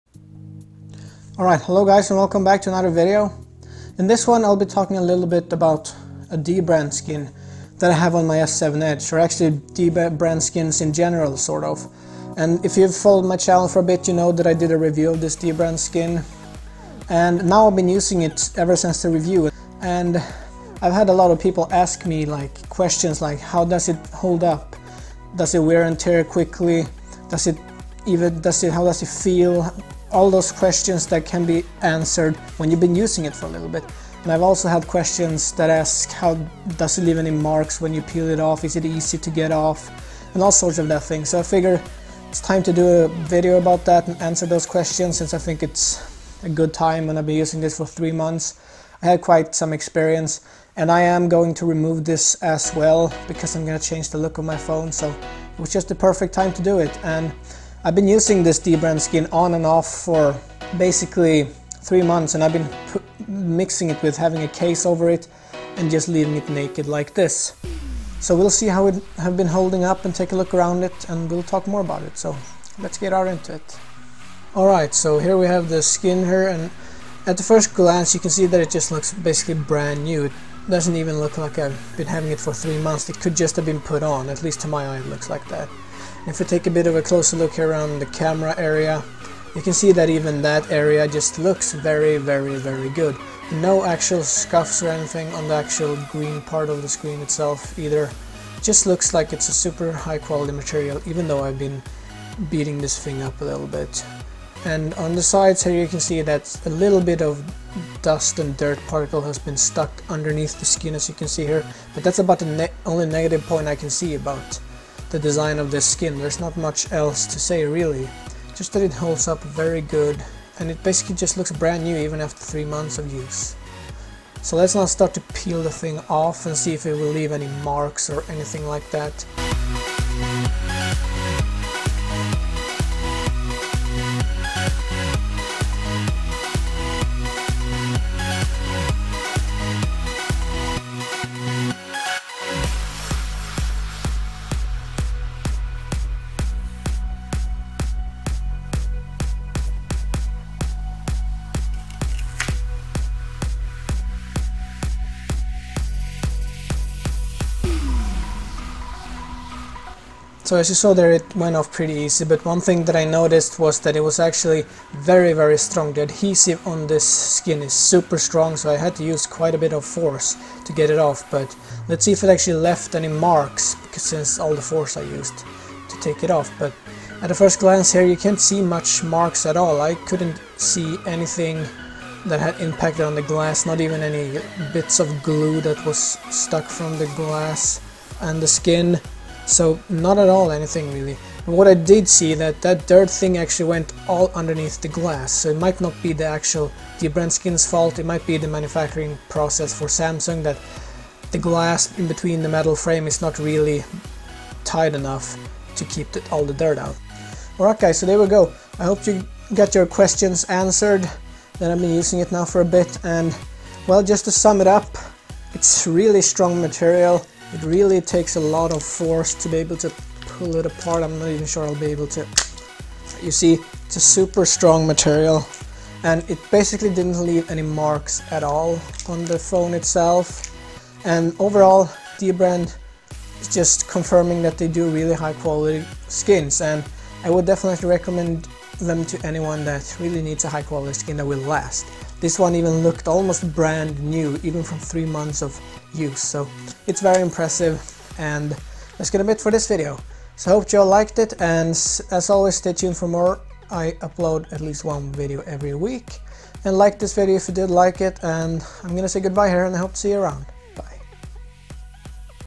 Dbrand Skin After 3 Months [Including Removal] | Does It Still Hold Up?
In this video I will take a look at how well a Dbrand skin will hold up after I have been using one on my S7 Edge for 3 months now. I will also be peeling it off to see if it will leave any marks on the actually device you put it on to.